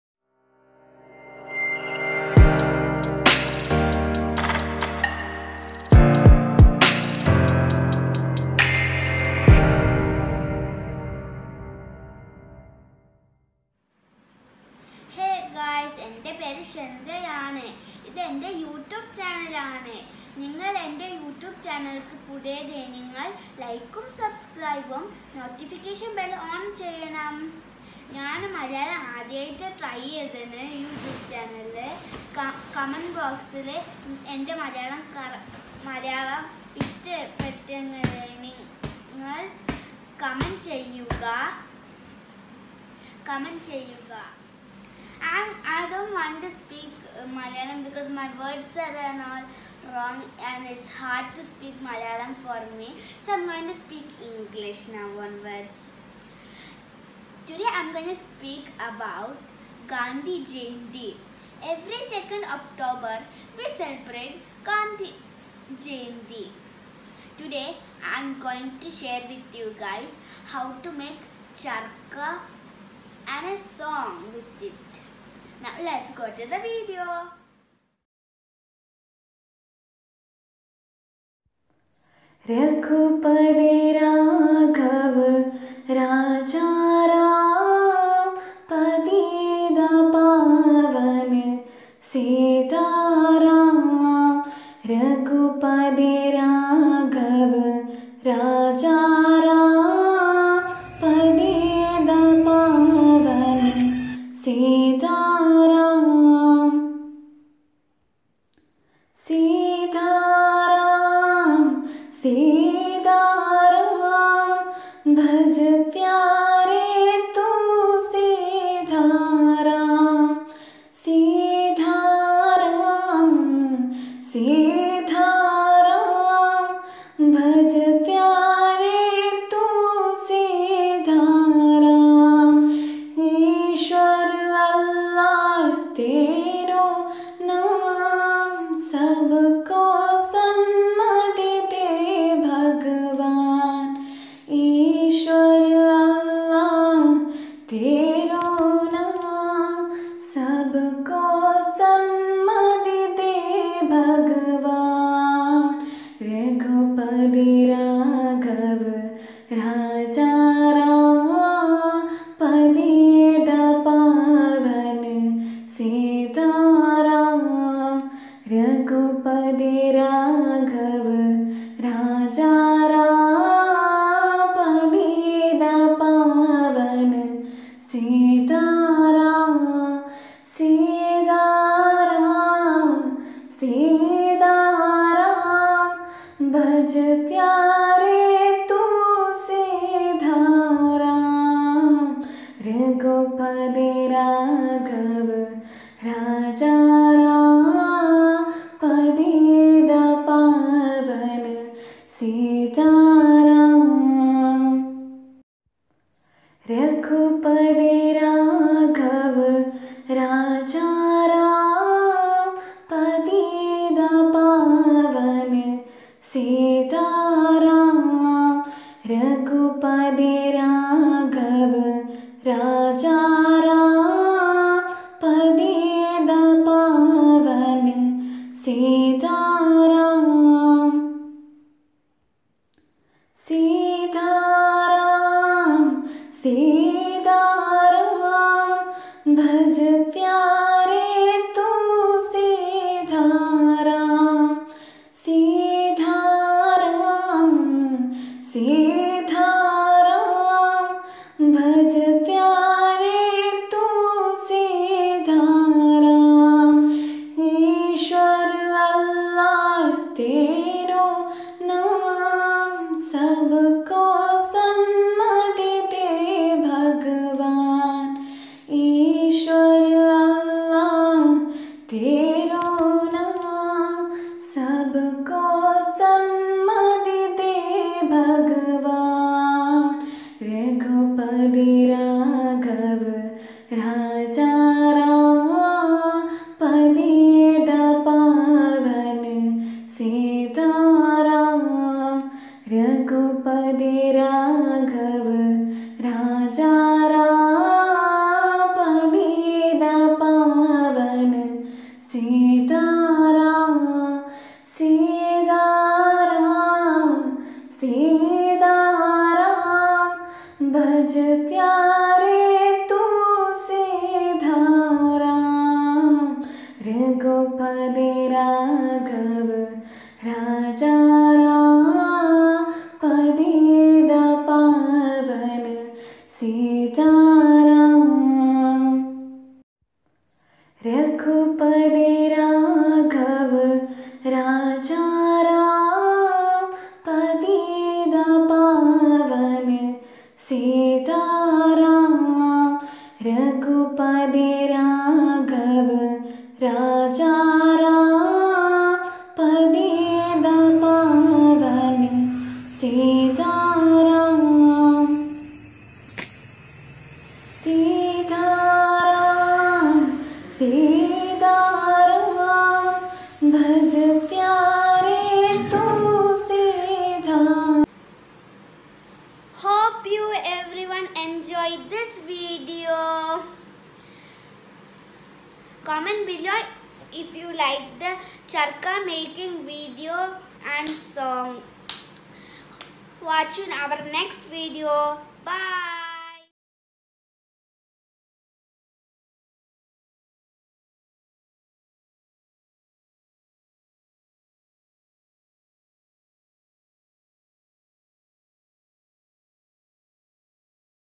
GANDHI JAYANTI SPECIAL VIDEO | CHARKHA MAKING|MY FIRST ATTEMPT IN MALAYALAM WAS FAILED |sweetie pie
Ghandi jayanti special video
Charkha making

Thambnail credit: india. com

Song by sis lulu
If you like this video drop ❤️
Comment also ✨

IMPORTANT :THIS CHANNEL IS OPERATING BY MY MOTHER